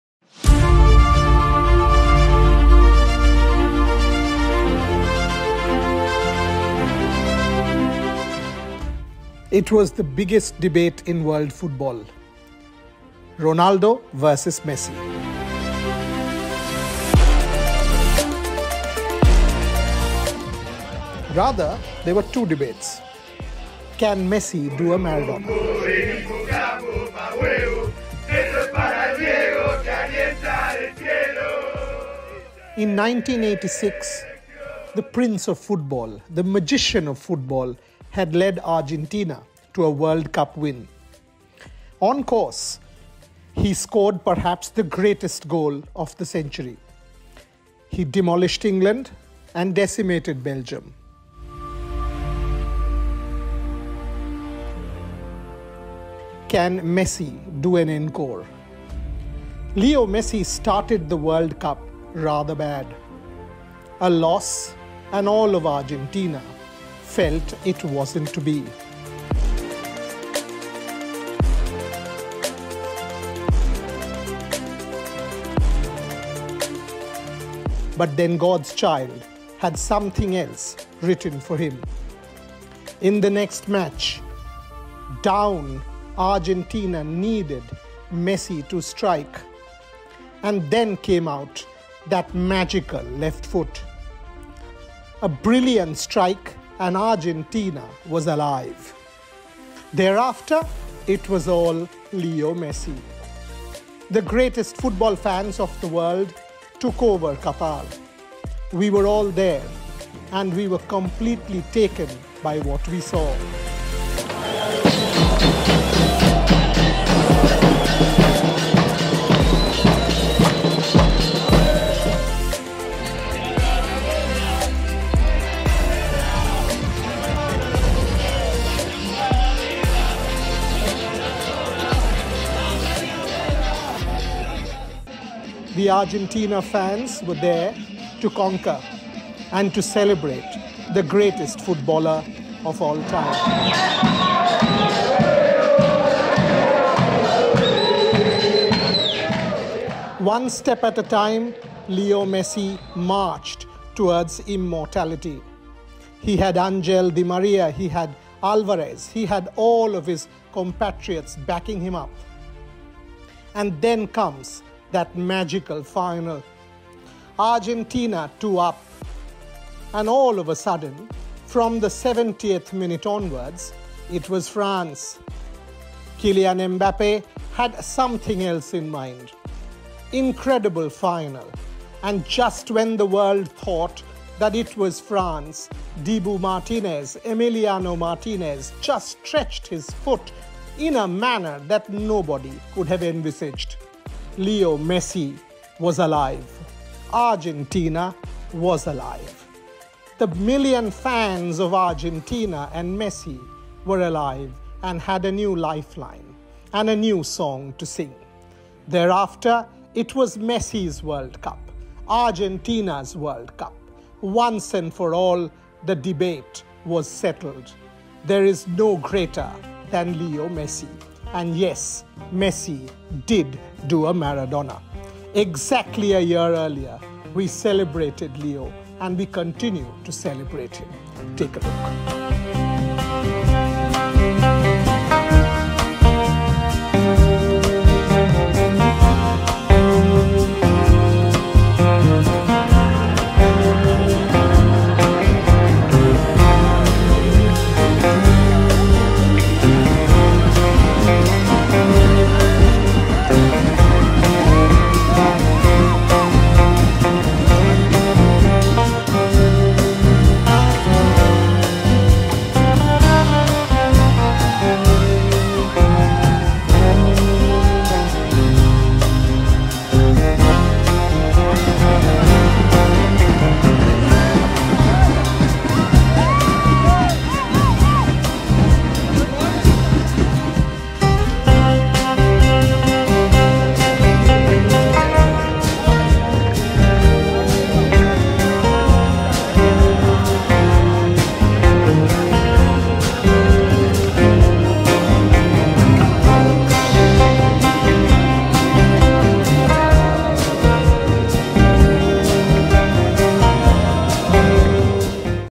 Looking back at that magical night in Qatar
OnThisDay last year, it was the crowning moment for LeoMessi.

@Argentina won the World Cup defeating France!

Qatar was painted blue, and millions of LaAlbiceleste fans celebrated. Team RevSportz covered it from the ground.

A look back at that magical day ❤